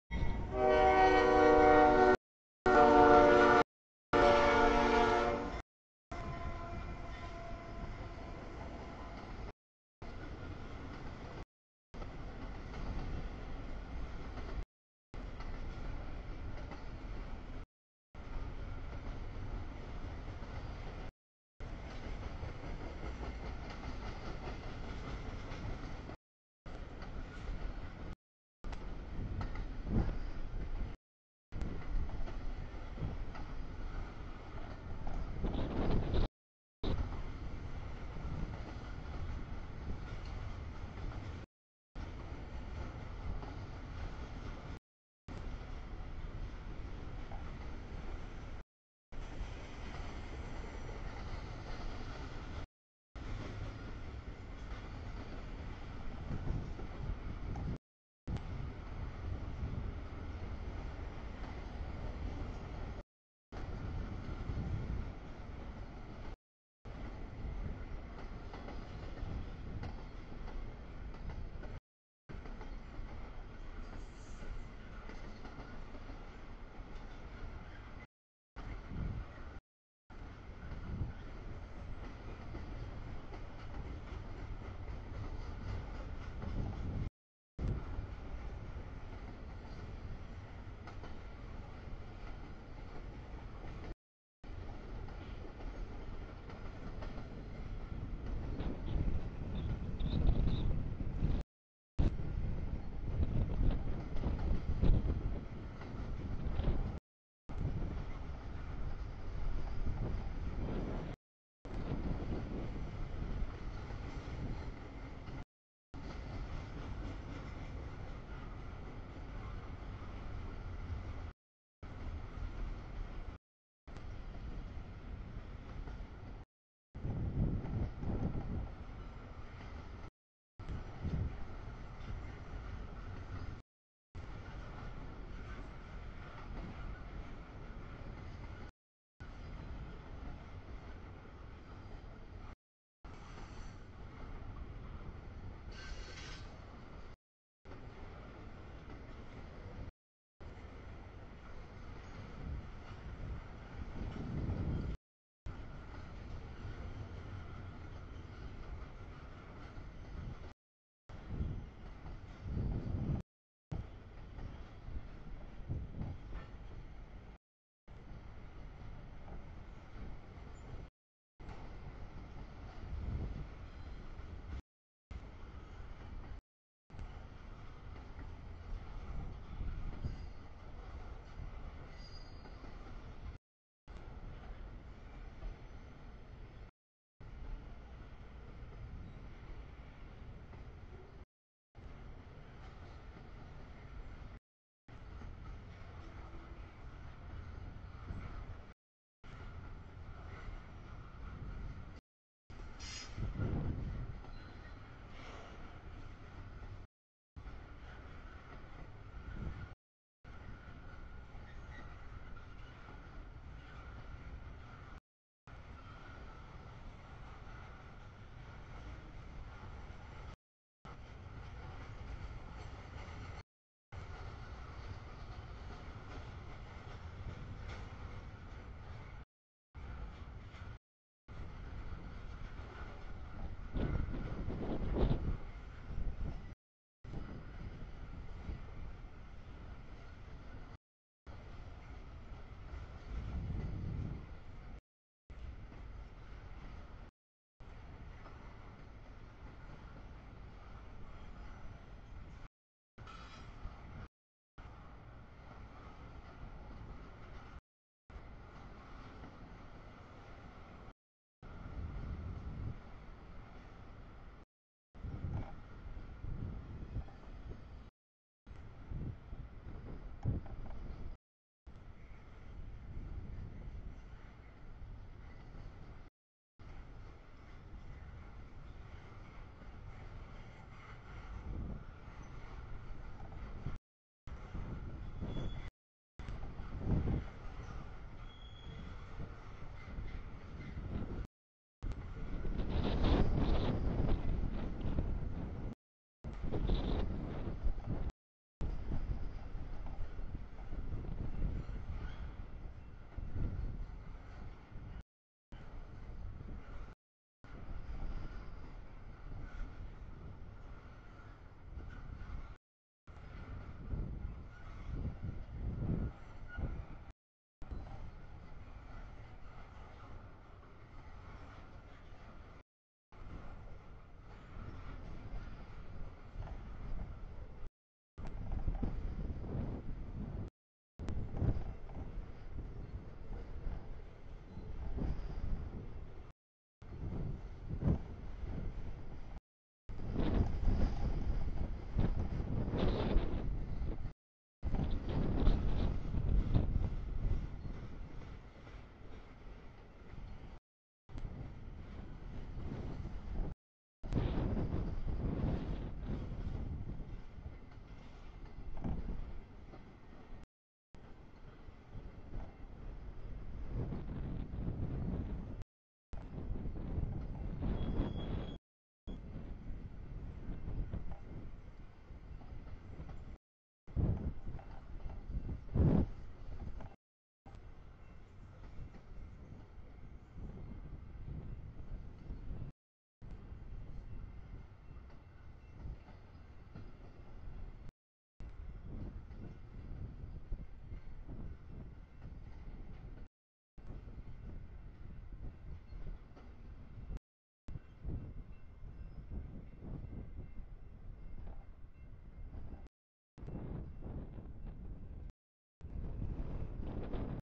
2- 9-2019 03:00 sb stack CSX Q162
00:26 sb stack CSX DPU Q004
01:00 nb stack trailers CSX Q009
01:17 nb stack CSX Q159
02:06 sb stack CSX Q160
02:14 nb auto rack CSX Q261
03:00 sb stack CSX Q162
05:48 sb stack trailers CSX Q010
06:12 nb mix CSX Q438
06:33 nb mix CSX 3101 DPU Q410
07:13 nb stack trailers CSX 3268, 743 Q003
07:33 nb stack trailers CSX 3263, 4033 Q007
09:22 sb stack trailers CSX 200, 242 Q008
09:29 nb auto rack CSX 709, 3229 Q253
11:24 nb nysw cut, mix CSX 781, 340, 5264 Q190
11:47 nb tanks BNSF 6957, 5310 BNSF 1119 K161
12:49 sb tanks BNSF 5140, 6846 K142
12:59 nb tanks CSX 5201, 335 K623
13:06 nb blue trash, mix CSX 64, 3079 Q434